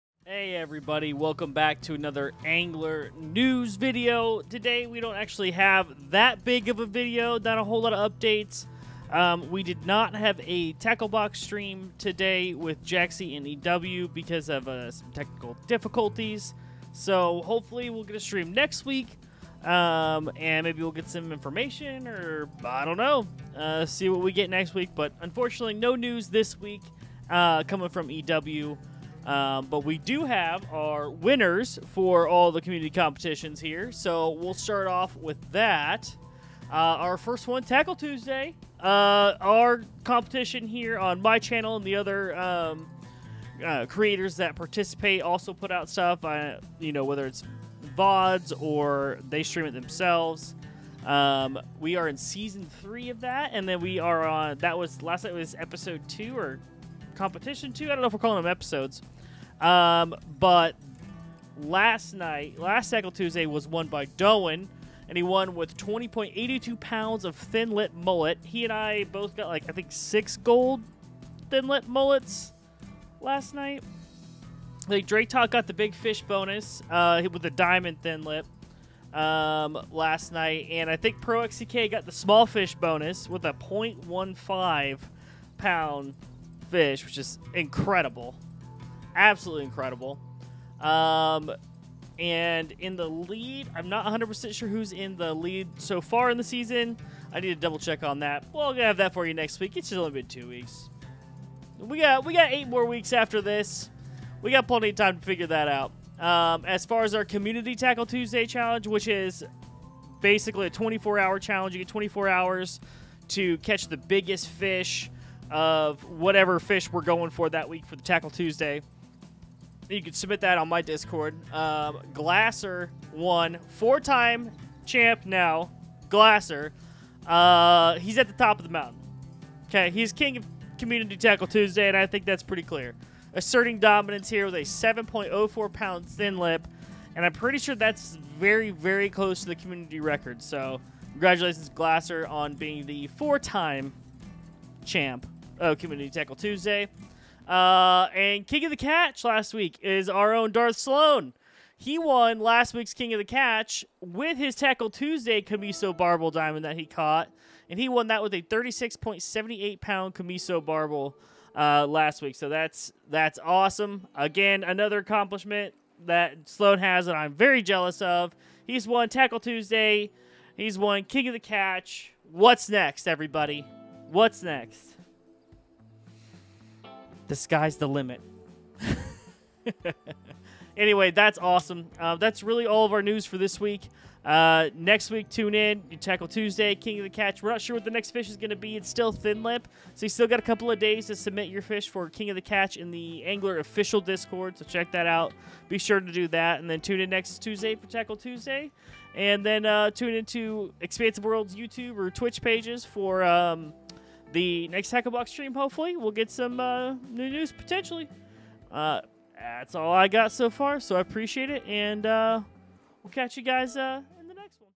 Weekly News Recap: Tackle Tuesday & Competition Winners!! | Call of the Wild: theAngler
There was no Tackle Box stream this week, so no news from Expansive Worlds on the game. But we do cover the competition winners!

If you realllllly enjoyed the video, consider joining my channel by becoming a member!